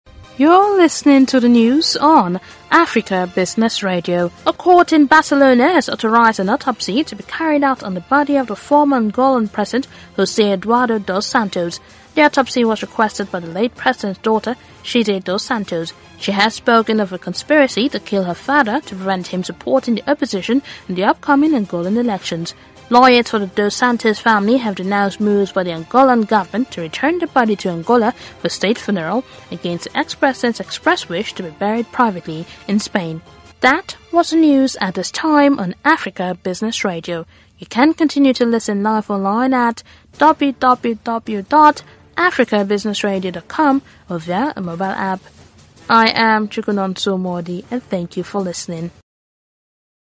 Spain Court Approves Autopsy On Angolan Ex-president
A court in Barcelona has authorized an autopsy to be carried out on the body of the former Angolan president, Jose Eduardo dos Santos.
The autopsy was requested by the late president's daughter, Tchize dos Santos.
She has spoken of a conspiracy to kill her father to prevent him from supporting the opposition in the upcoming Angolan elections.
Lawyers for the Dos Santos family have denounced moves by the Angolan government to return the body to Angola for a state funeral, against the ex-president's expressed wish to be buried privately in Spain.